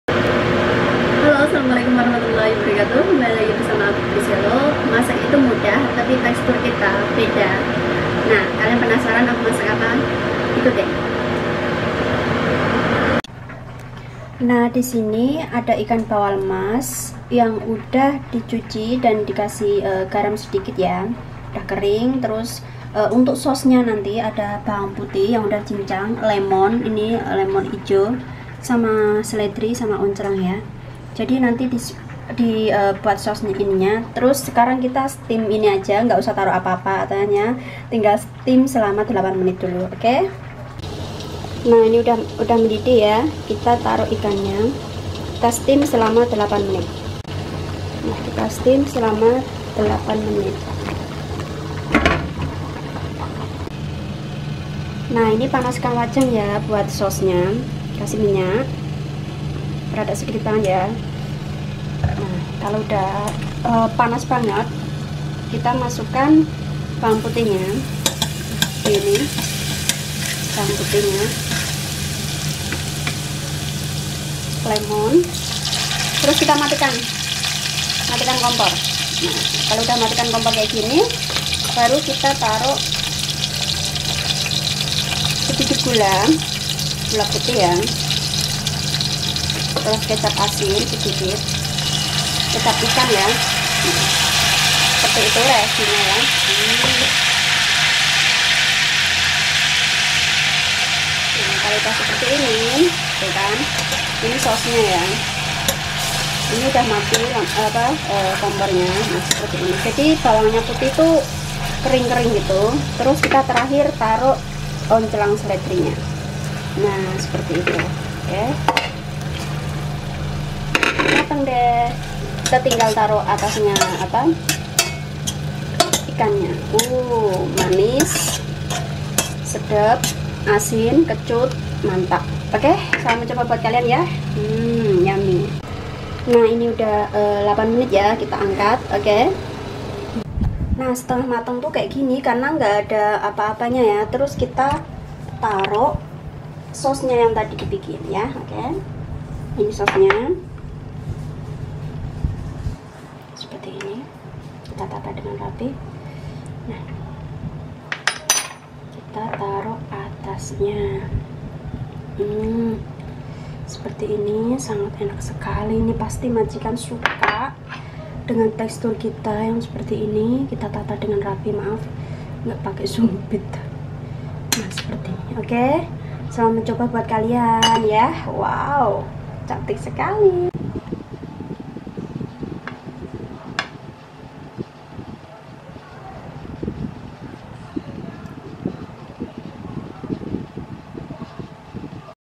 RESEP STEAM IKANBAWAL EMAS ala tkw hongkong
assalamualaikum kk
hari in steam ikan bawal y yg mudah DENGAN trik chinese yg jg sehat

bahan:
ikan bawal emas
bawang putih cincang
lemon 2/3 slice
daun bawang

bumbu:
garam
gula kecap ikan/asin

sausnya:
siapkan panci ksi minyak smpai panas trs matikan kompor.masukan bawang putih lemon trs ksi gula Dan kecap asin trs daun bawang aduk sisihkan

caranya:
cuci ikan smpai bersih Dan lap bersih..tata ikan dipiring unk steam ..
setelah matang tuang sausnya diatas ikan merata siap hidangkan mudah sekale

tips: style Chinese adalah masakan yg pling sehat krn dengan rempah bumbu yy simple tp kenikmatanya sangt nyata..

nb:salam satu hoby
I lope you all
selamat mencuba
semoga bermanfaat